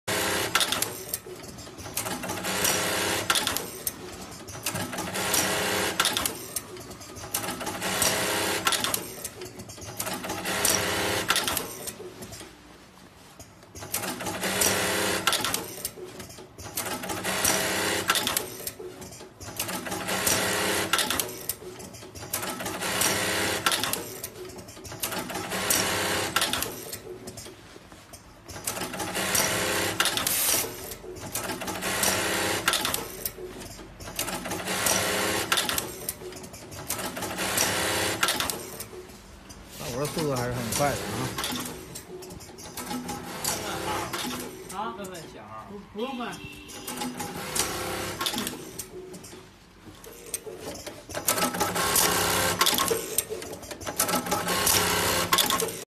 SAKURA-STITCH S-254 Fully Automatic Two Needle Belt Loop Attaching Machine
This S-254 is mainly used for attaching jeans and casual pants belt loop. It is simple and easy to operate. Processes of automatic checking, cutting, folding and tacking are completed in one time. It offers flexible adjustment, and high efficiency production.